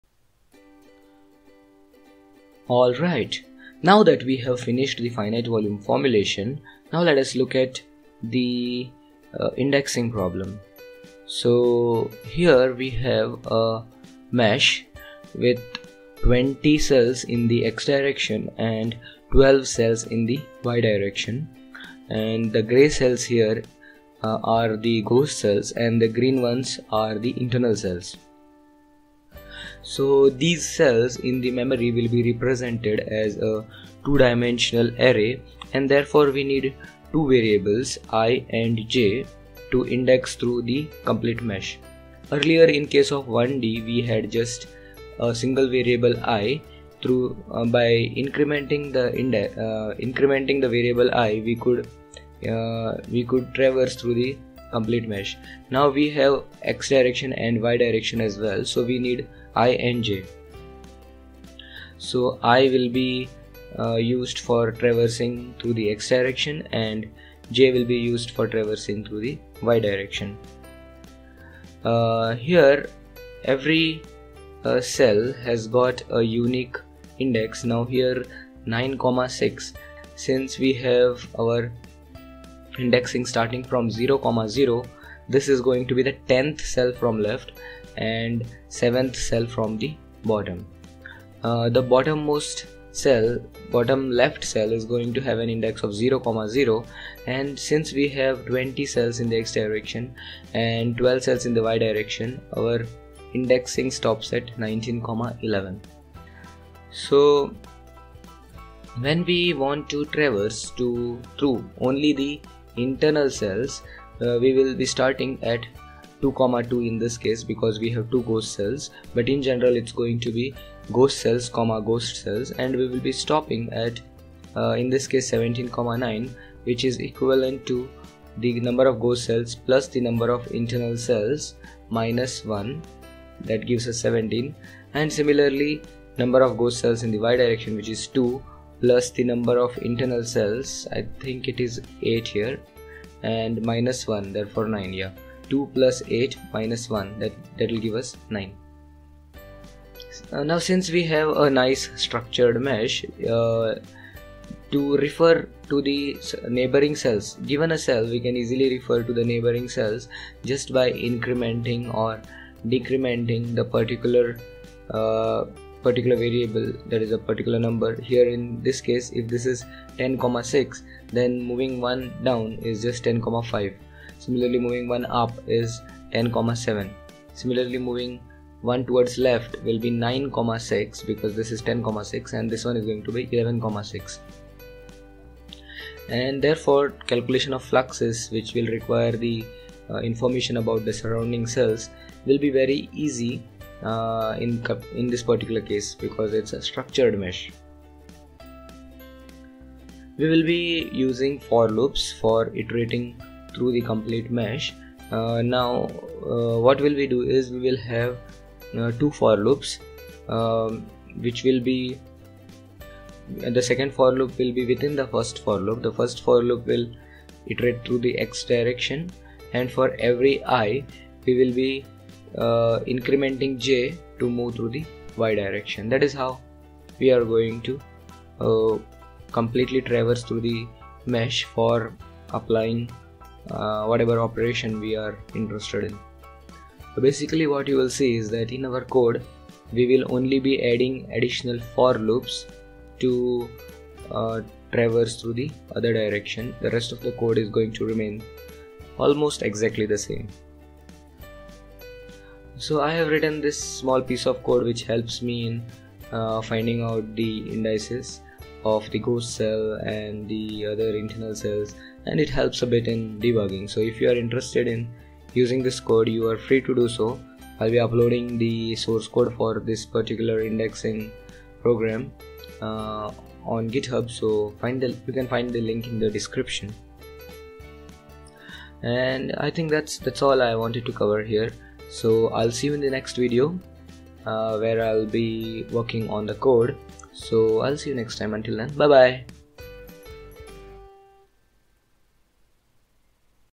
02 - Indexing through 2D Cartesian Mesh
This is a video where I provide an overview of the mesh that will be used and the kind of indexing that we will have for ghost and internal
cells in the code for 2D Cartesian problem.

where I write about numerical methods and other things that interest me.